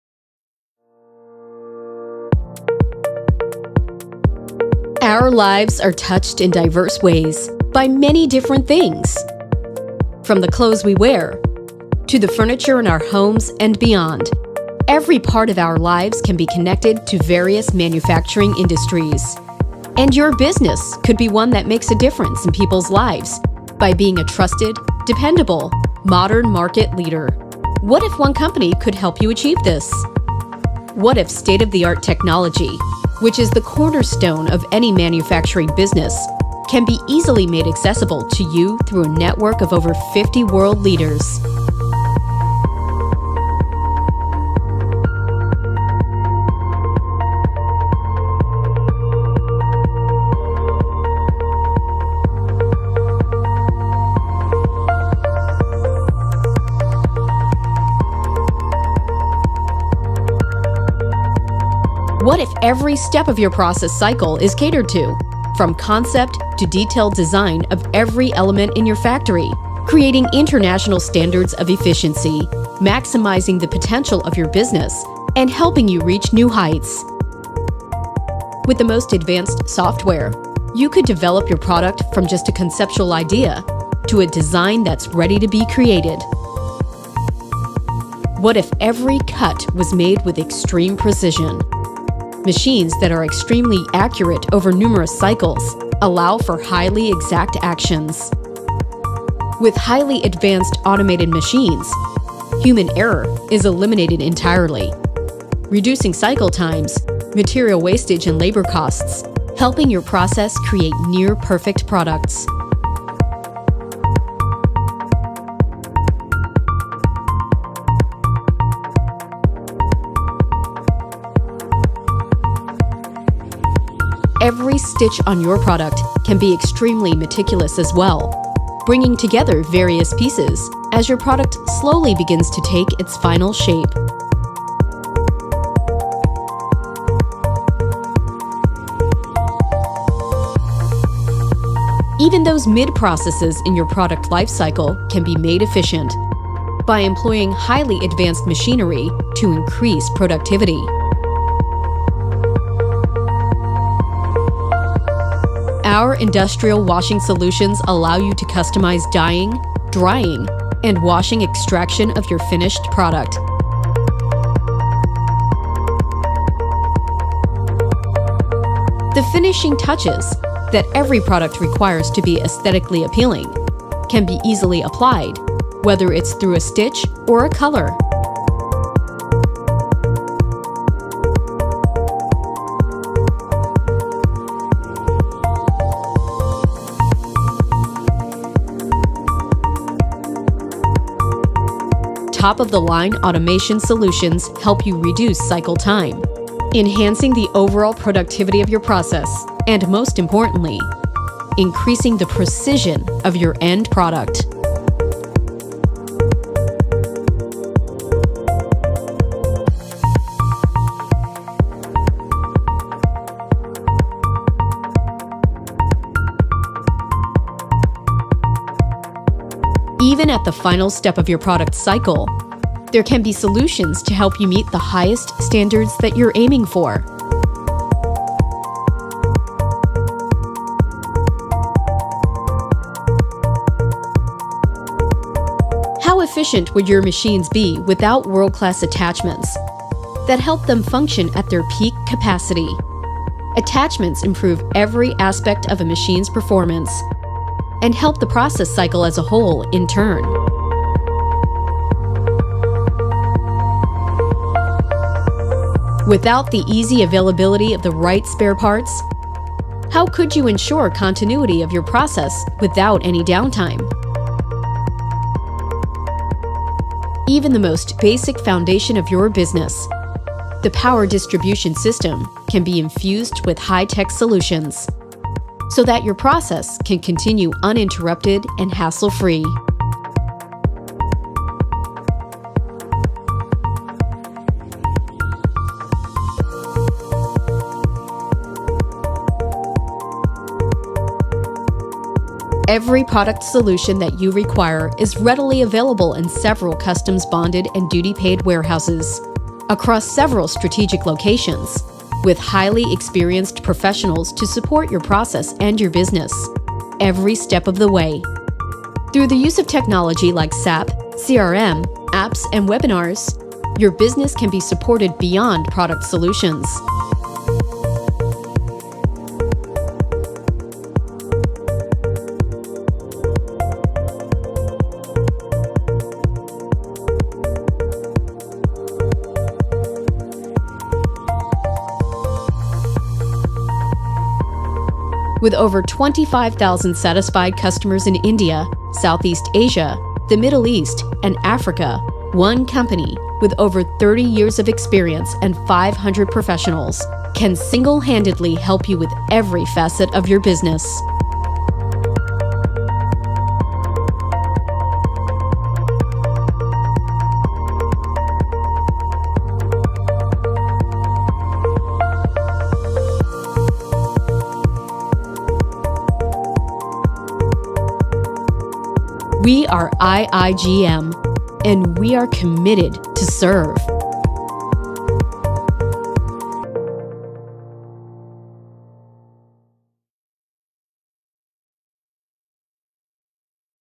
IIGM Corporate Film - Garment Industrial Machines, Spare Parts & Services
India's foremost technology and service provider IIGM Pvt. Ltd with the network of 45 world leaders in home textiles and apparel technology spread across many continents, 15 offices across India and 3 offices abroad - Bangladesh, Ethiopia, Jordan & employing over 500 professionals with over 25,000 satisfied customers, all in a span of 3 decades, speaks volume about the success and growth record. We use technology in every facet of our business by using SAP, CRM, Apps and Webinars to enhance deliverables and satisfy user experience. Our core team manages each vertical with a dedicated group of sales, application and service professionals. IIGM has the ability to serve clientele across the spectrum from small companies to large scale multinational brands.